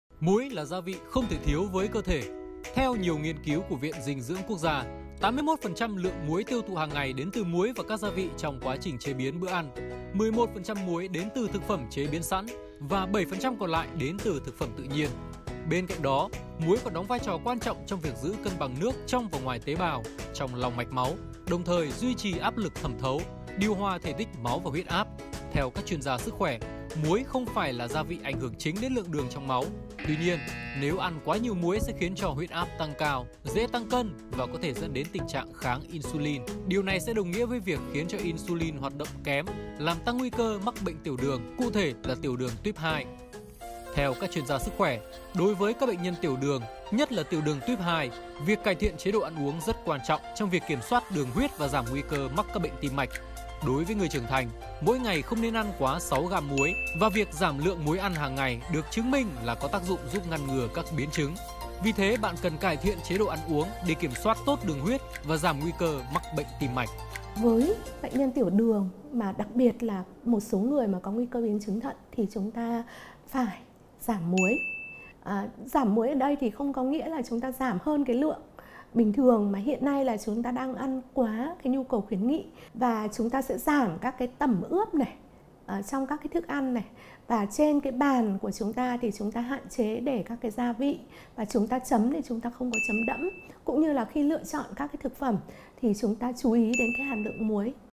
TĂNG NGUY CƠ MẮC BỆNH TIỂU ĐƯỜNG DO ĂN NHIỀU MUỐI
Muối và đường, về hương vị thì có vẻ không liên quan đến nhau nhưng ăn quá nhiều muối thì có thể là tác nhân tăng nguy cơ mắc bệnh tiểu đường đó nha!
Xem thêm các chương trình đặc sắc của VTV3 tại:
Fanpage: